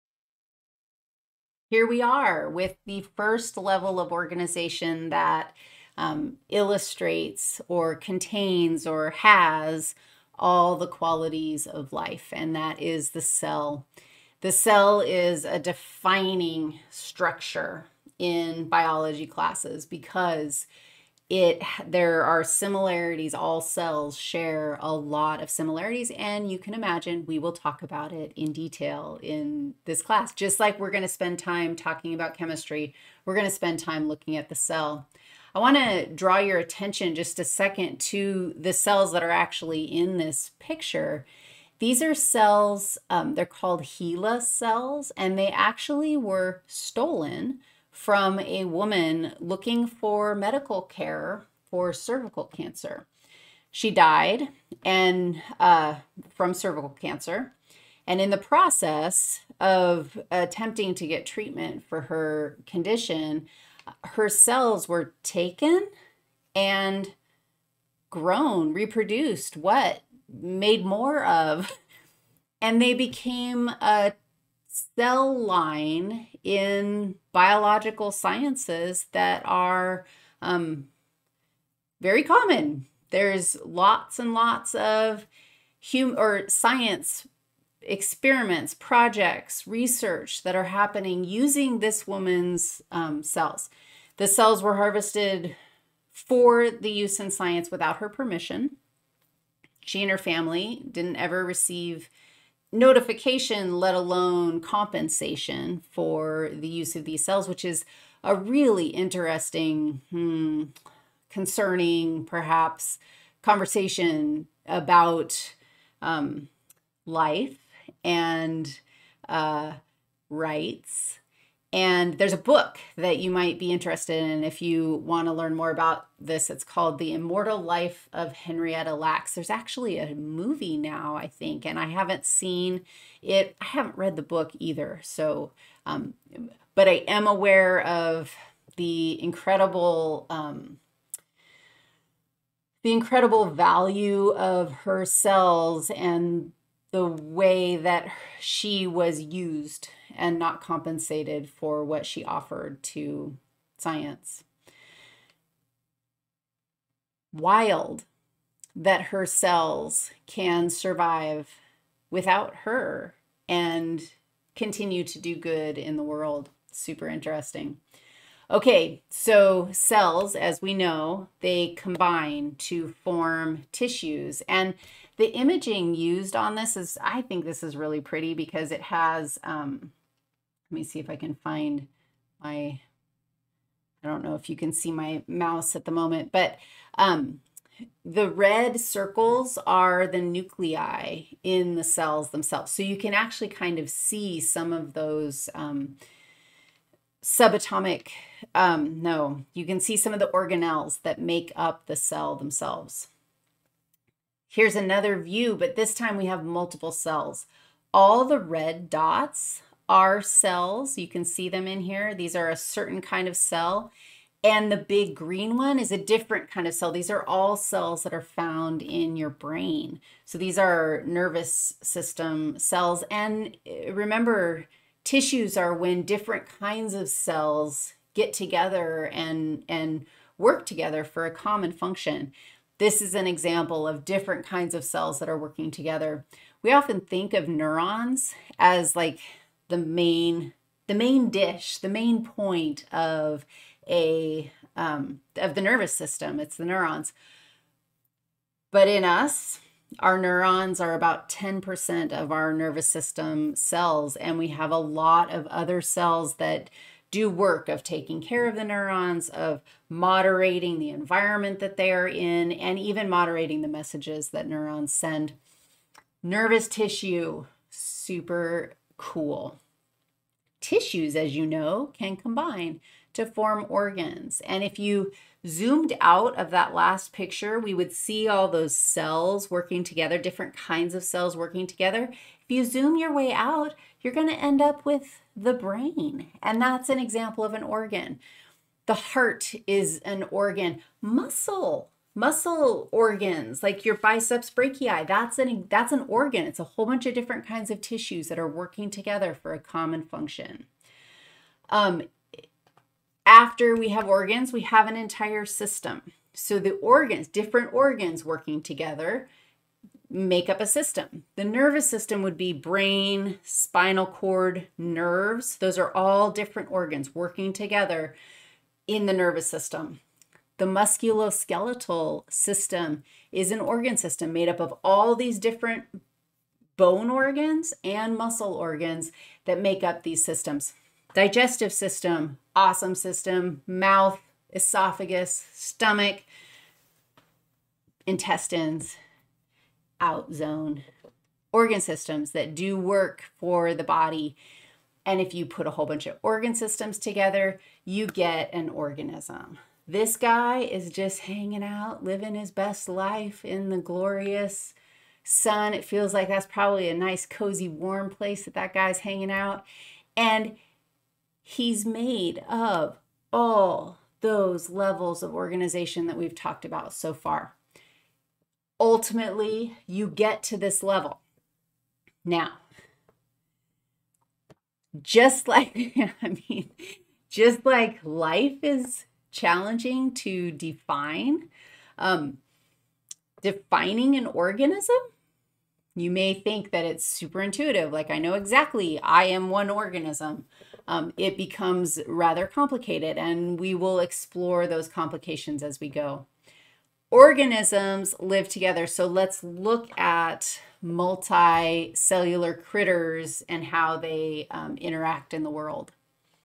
Life 3b- Multicellular critters
Part 5 of 8 in a lecture on "Life" for a non-majors General Biology course. You can watch all 8 video clips by going to the PLAYLIST